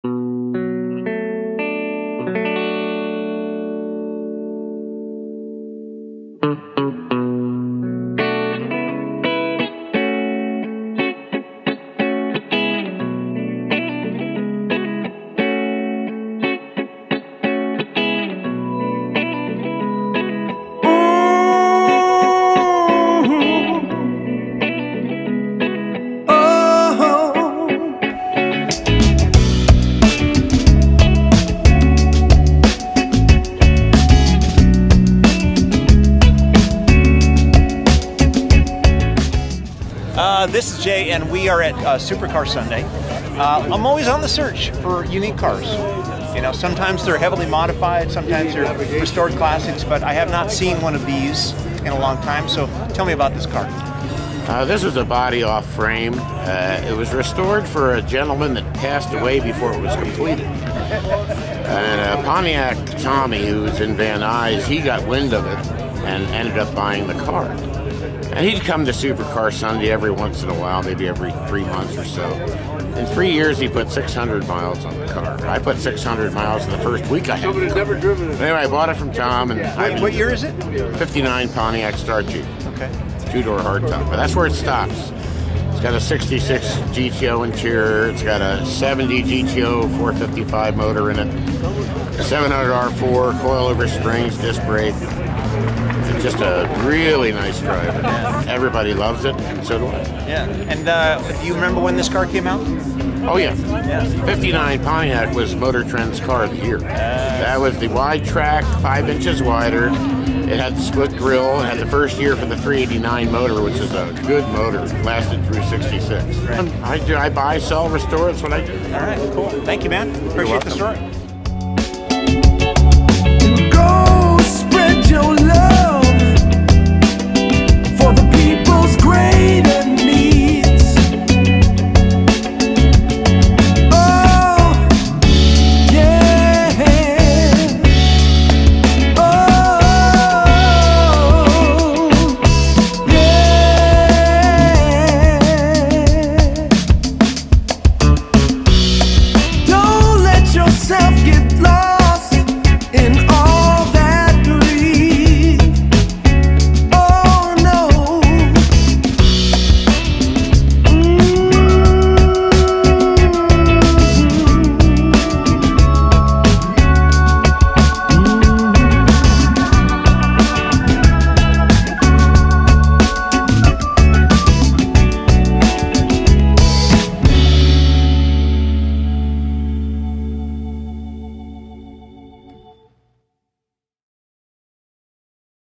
1959 PONTIAC STAR CHIEF FULLY RESTORED- 3MINUTE KUSTOMS
- 1959 PONTIAC STAR CHIEF FULLY RESTORED- 3MINUTE KUSTOMS - Fireball meets up with JR Noll and his flawless 1959 Pontiac Star Chief at Supercar Sunday.

1959 PONTIAC STAR CHIEF FULLY RESTORED- 3MINUTE KUSTOMS

1959 PONTIAC STAR CHIEF FULLY RESTORED- 3MINUTE KUSTOMS